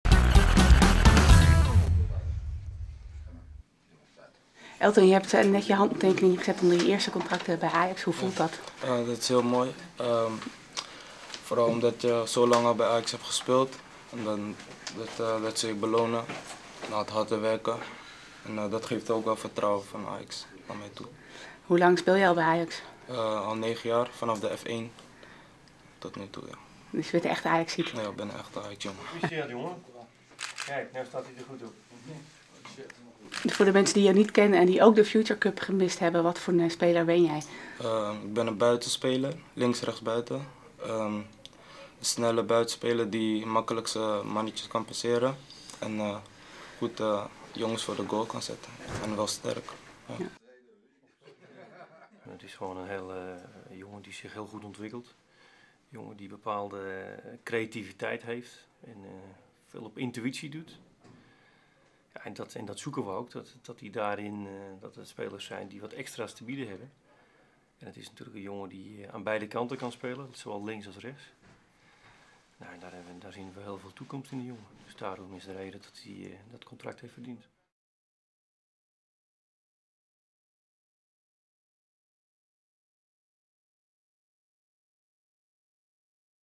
Acolatse: 'Beloning voor hard werken'
Elton Acolatse tekende vrijdag zijn eerste profcontract. De buitenspeler van de B1 bekroont daarmee een succesvol seizoen waarin hij de AEGON Future Cup en het landskampioenschap won.